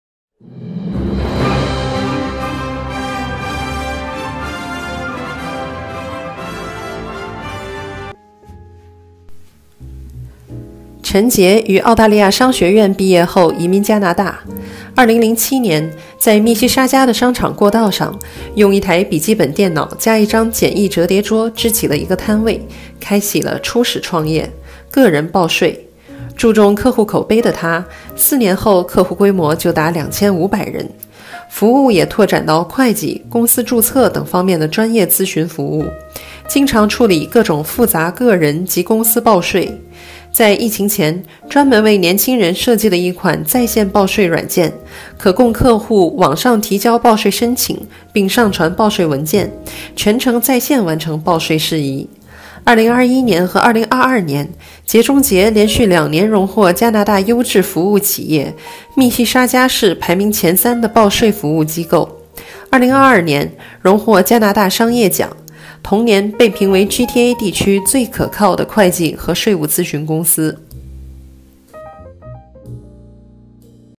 Top Women CEO Award - Jie Chen
陈杰于澳大利亚商学院毕业后移民加拿大。2007年，在密西沙加的商场过道上，用一台笔记本电脑加一张简易折叠桌支起了一个摊位，开启了初始创业——个人报税，注重客户口碑的她，4年后客户规模就达2500人。服务也扩展到会计、公司注册等方面的专业咨询服务，经常处理各种复杂个人及公司报税。在疫情前，专门为年轻人设计了一款在线报税软件，可供客户网上提交报税申请并上传报税文件，全程在线完成报税事宜。2021年和2022年，杰中杰连续两年荣获加拿大优质服务企业、密西沙加市排名前三的报税服务机构。2022年，荣获加拿大商业奖；同年被评为GTA地区最可靠的会计和税务咨询公司。

Jie Chen immigrated to Canada after graduating from business school in Australia. In 2007, she set up a booth in a shopping mall in Mississauga. Using a laptop computer and a simple folding table, she started her first business – providing individual tax filing services. Paying high attention to customer experience, within four years Jie grew her business to serve up to 2,500 customers. The services Jie provides have expanded to include professional consulting services in accounting and company registration, and her company regularly handles complex personal and corporate tax returns. Before the pandemic, Jie designed an online tax filing software especially for young people that allows customers to submit tax filing applications and upload tax filing documents, to complete the entire tax filing process online. In 2021 and 2022, Triple J was recognized as one of Canada's Quality Service Enterprise for two consecutive years, and as one of the top three tax preparation service agencies in Mississauga. In 2022, Triple J won the Canadian Business Award, that same year, it was named the most reliable accounting tax consulting company in the Greater Toronto Area.